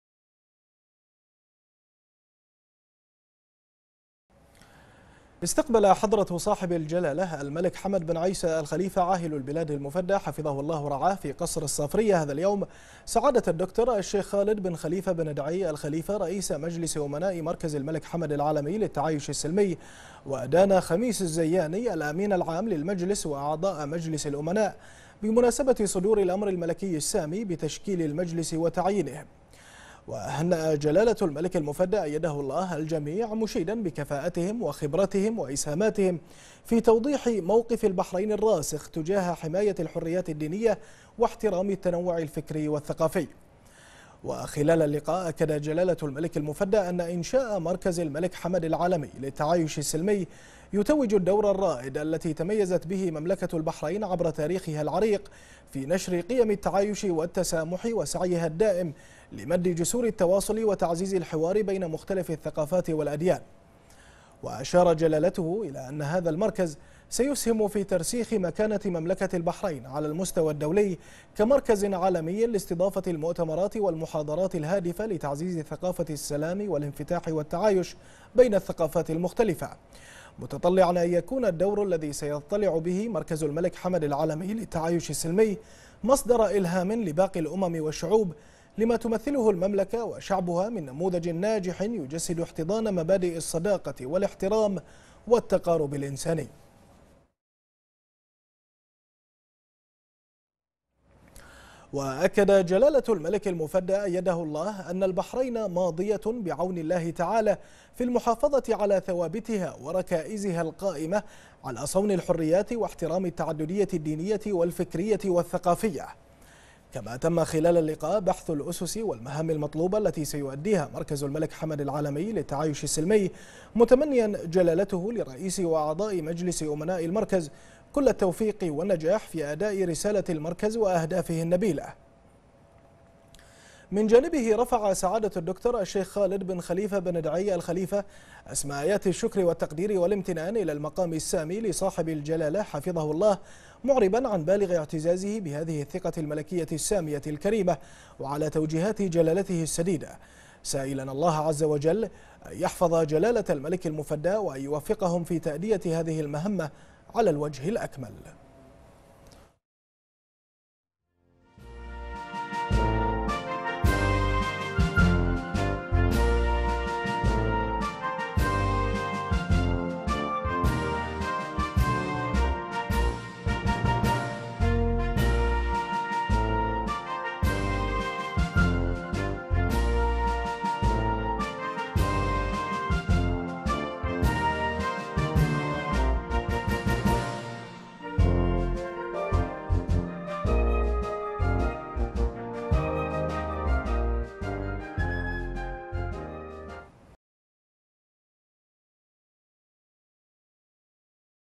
البحرين : جلالة الملك المفدى يستقبل رئيس مجلس أمناء مركز الملك حمد العالمي للتعايش السلمي
جلالة الملك المفدى يستقبل رئيس مجلس أمناء مركز الملك حمد العالمي للتعايش السلمي